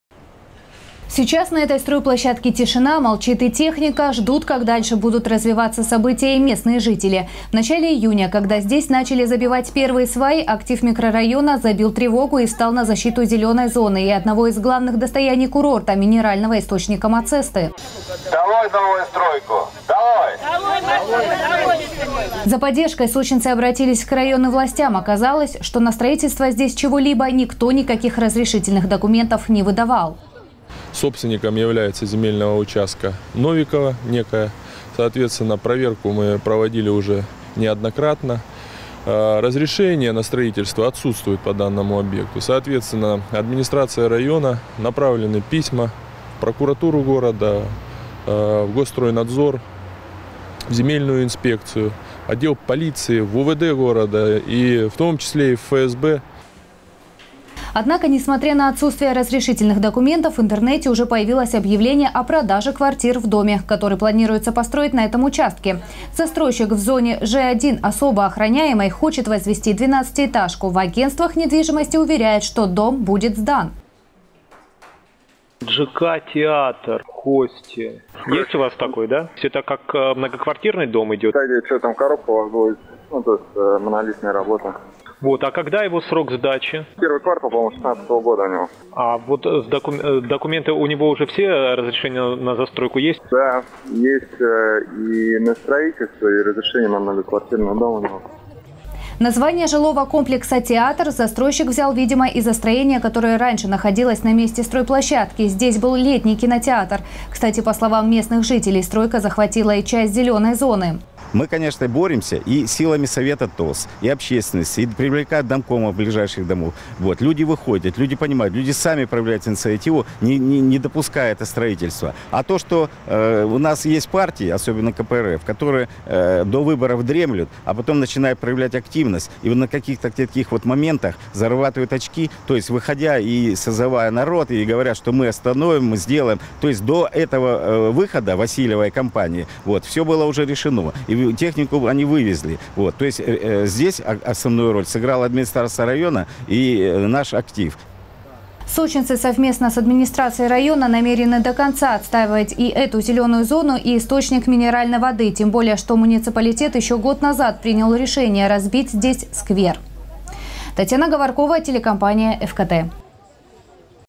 Строительство дома в Хостинском районе Сочи- незаконно. Новости Сочи Эфкате
Сегодня стали известны новые подробности о незаконной стройке в Хостинском районе на улице Октября. В начале июня администрация района вместе с жителями Хосты встали на защиту зелёной зоны, а главное — уникального минерального источника, над которым коммерсанты решили возвести строение. Для этого они стали забивать 10 метровые сваи.
Сочи. Новости.
Телеканал Эфкате.